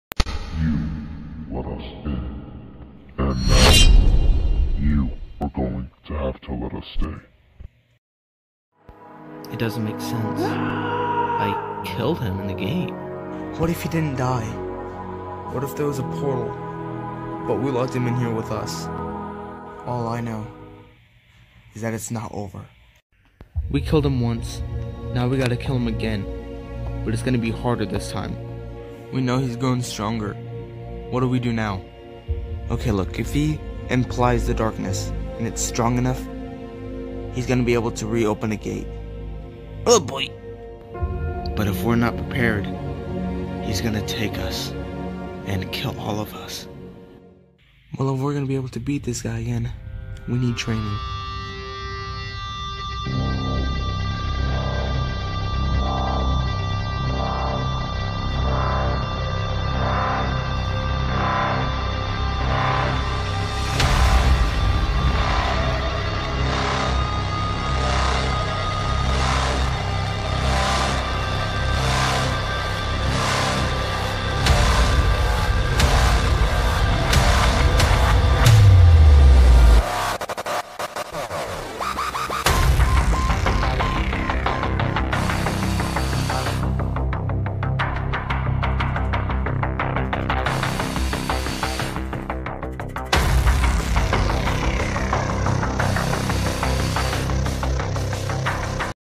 The Godzilla's Season 7 the awakening trailer
up for another season, well you got one. Season 7 is coming out on October 15th so be ready